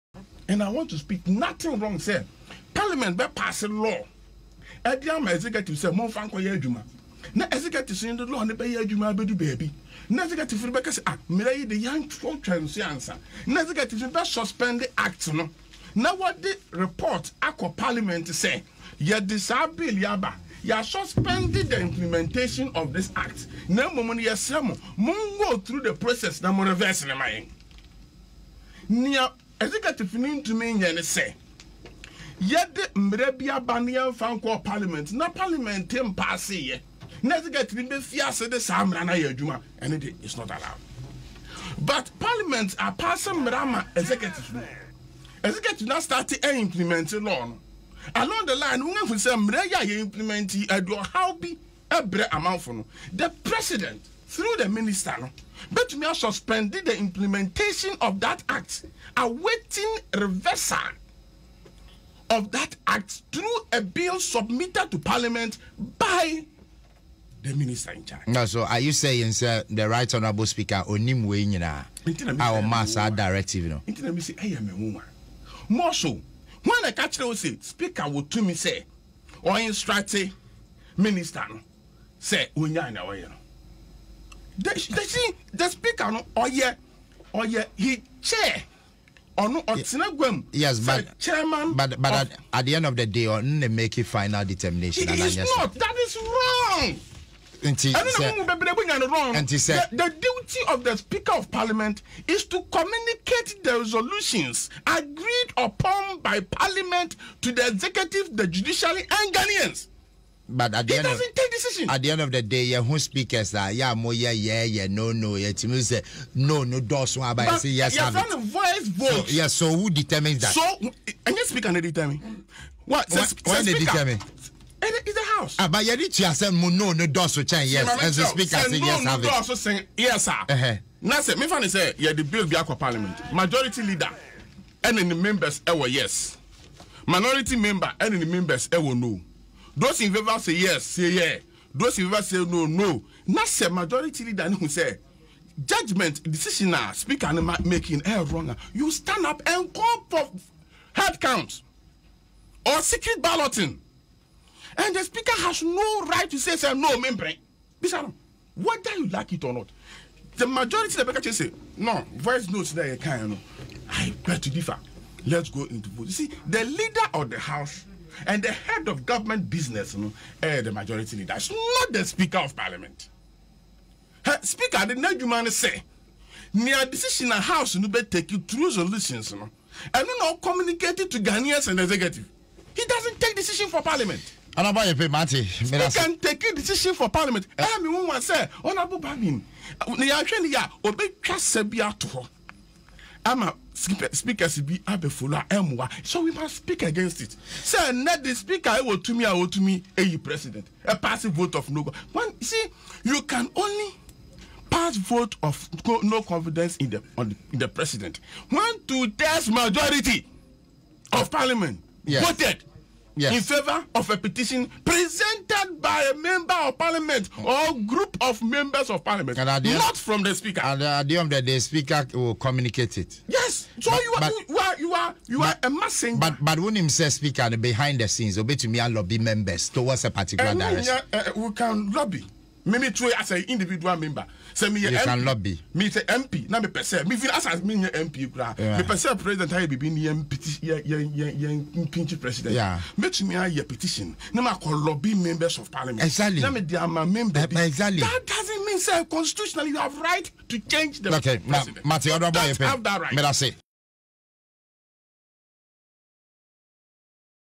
Speaker of Parliament has no power to instruct ministers on behalf of the legislature - NPP
Speaker of Parliament has no power to instruct ministers nor make decision on behalf of the legislature - Samuel Ayeh-Paye, Former MP - NPP